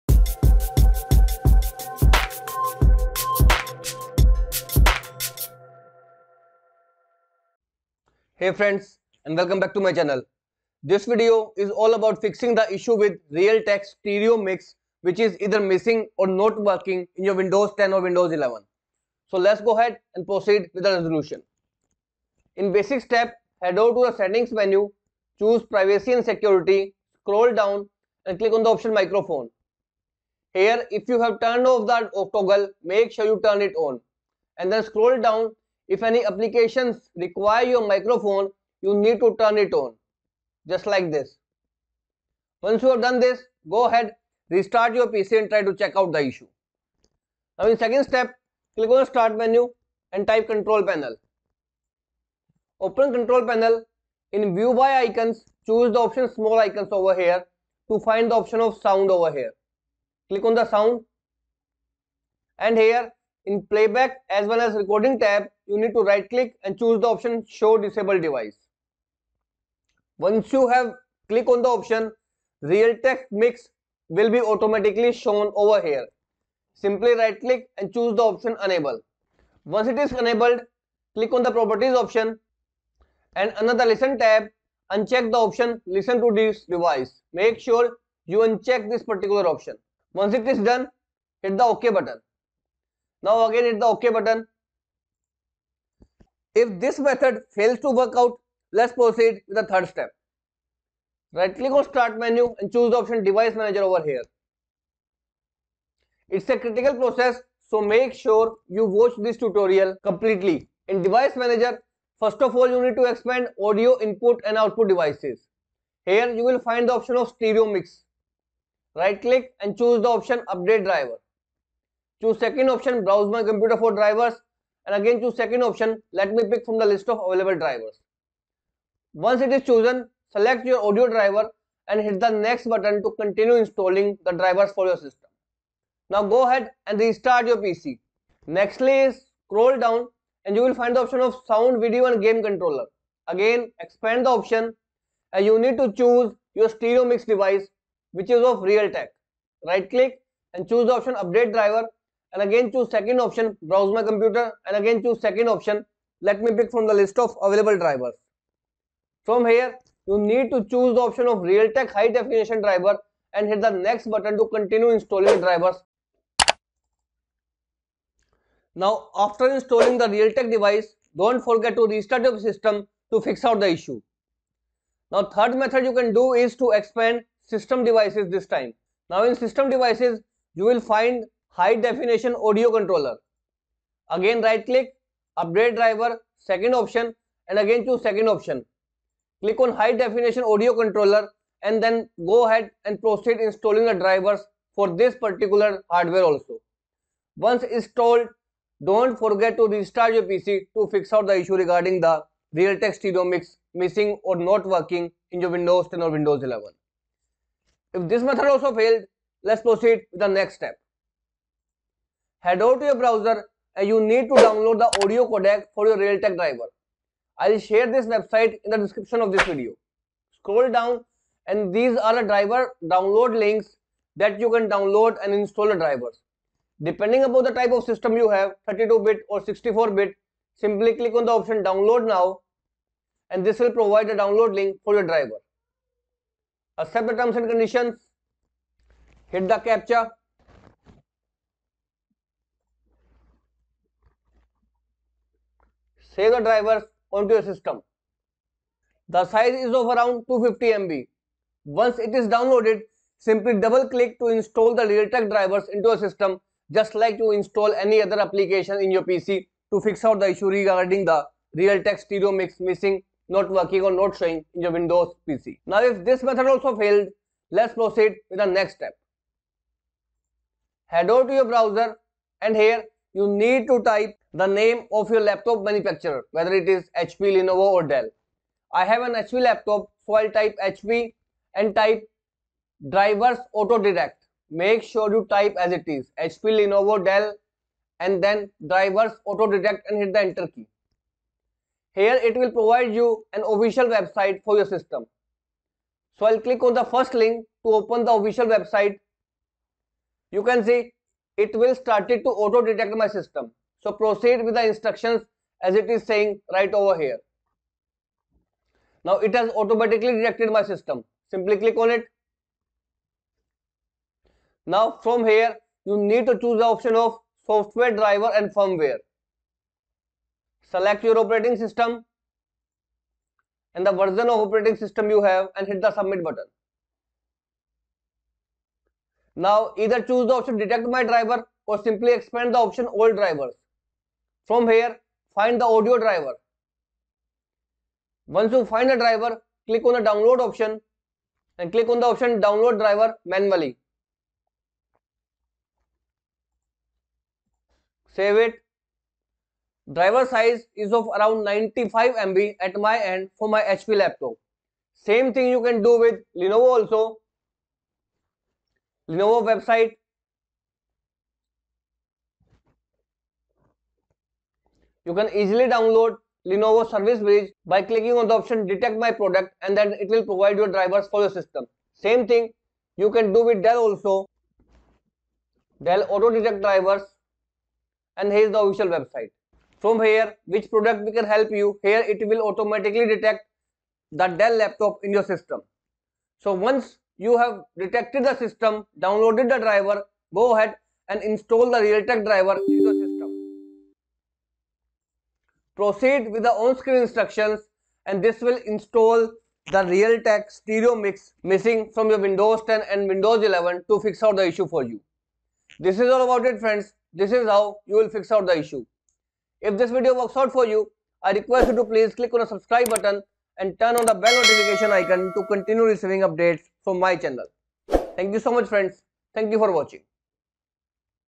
Realtek stereo mix missing not working
Realtek stereo mix missing or not working issue is majorly arise in windows 11 due to availability of more than one sound device in the system which conflict with the realtek devices and cause to be the stereo mix to disable or its not working at all. So this video is ablut fixing the issue with realtek stereo mix not working in windows 11 or missing from the sound devices in windows 11

00:00 Introduction
00:22 Method 1 using settings
00:48 Method 2 using control panel
01:36 Method 3 using device manager
03:28 Method 4 using realtek codec
04:30 Method 5 using manufacturer website